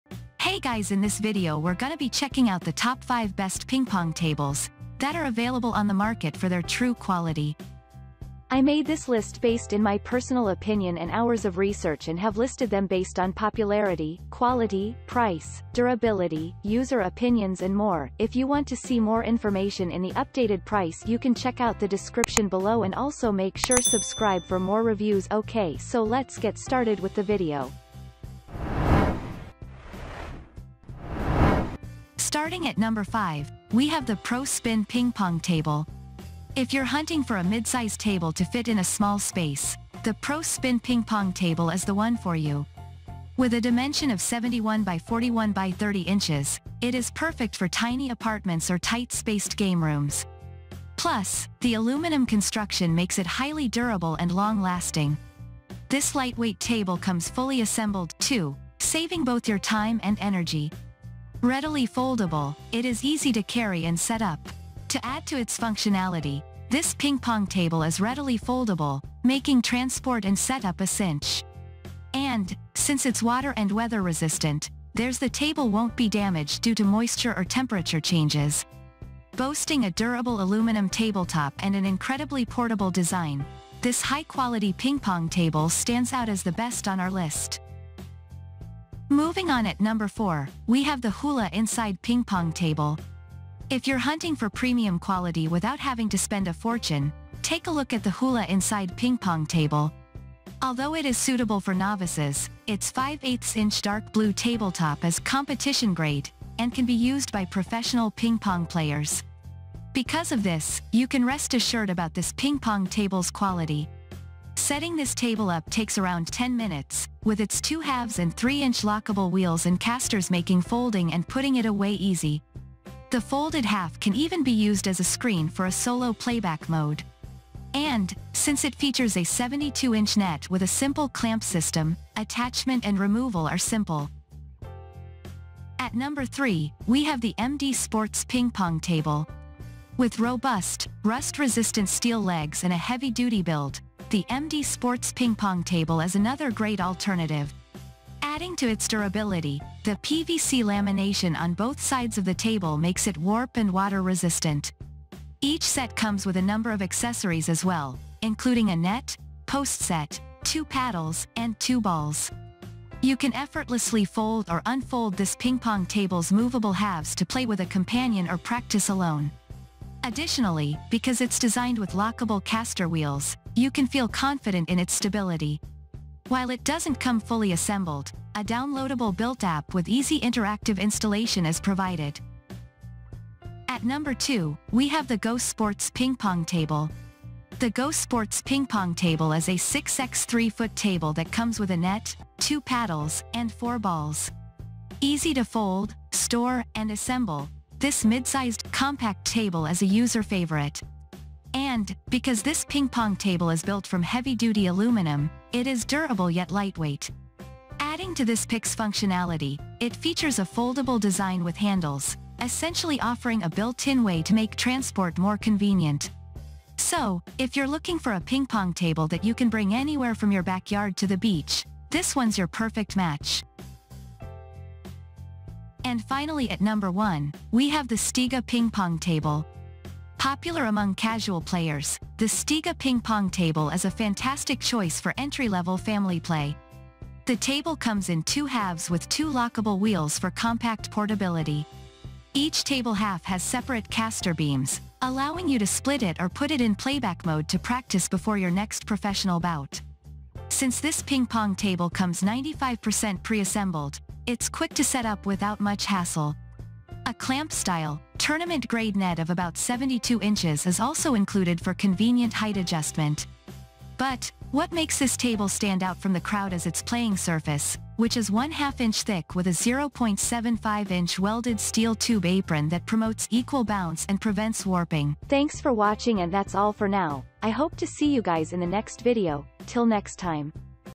Top 5 Best Ping Pong Tables in 2024
Check Price on aliexpress

▶️ 5. PRO-SPIN Ping Pong Table

▶️ 4.JOOLA Inside Ping Pong Table

▶️ 3.MD Sports Ping Pong Table

▶️ 2. GoSports Ping Pong Table